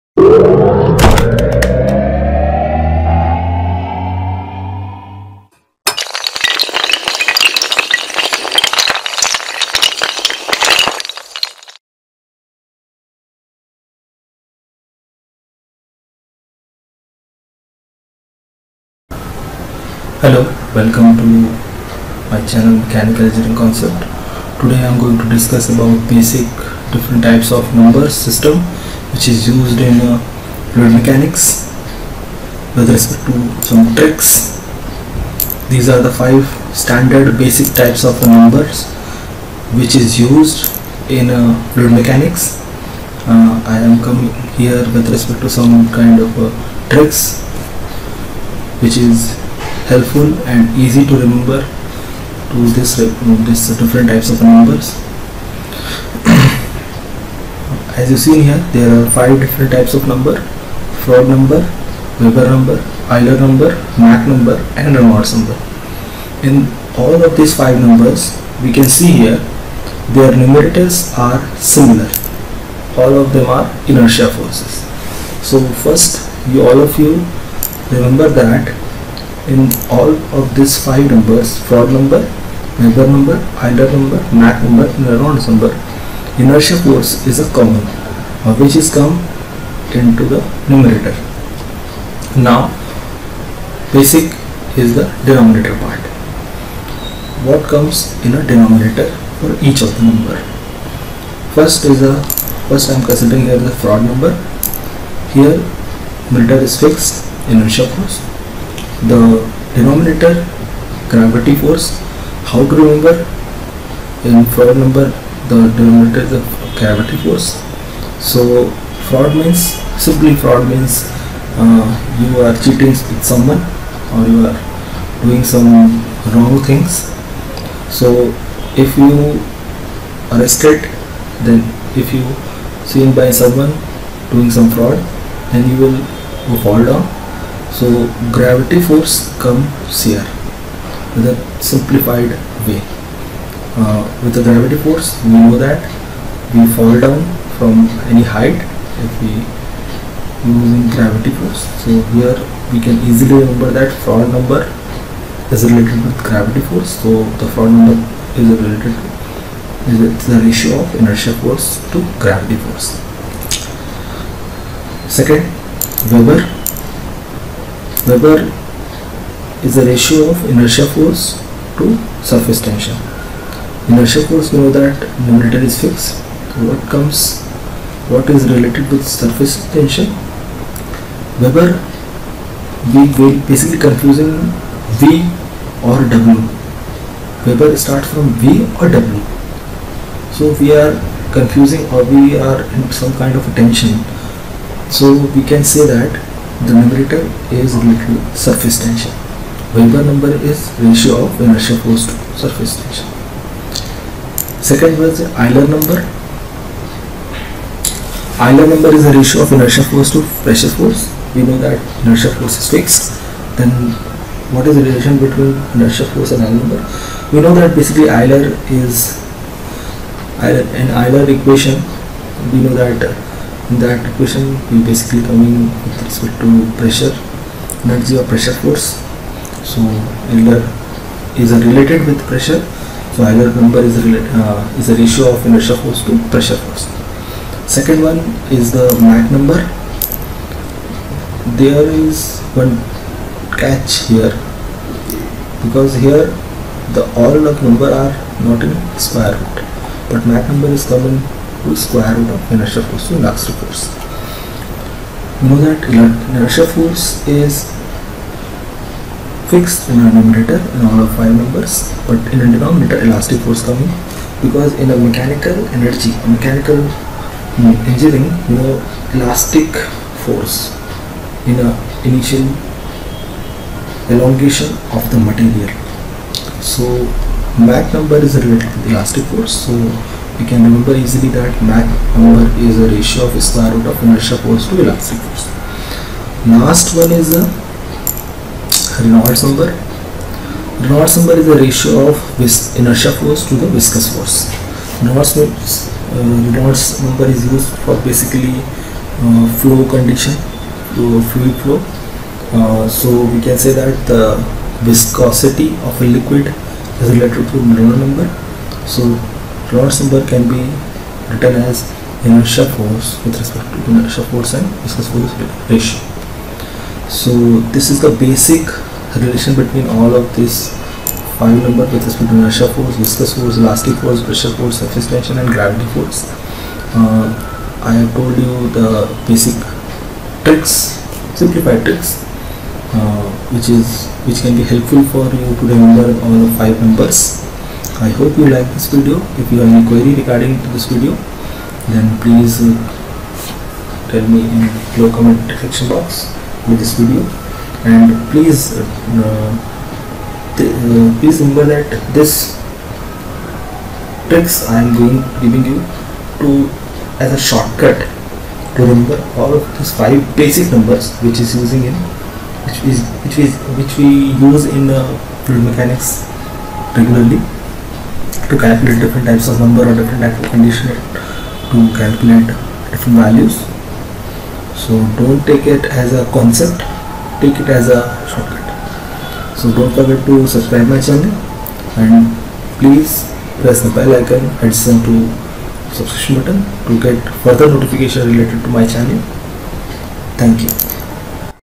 Fluid mechanics numbers short-cut tricks
Detail concept and shortcut trick to remember numbers of fluid mechanics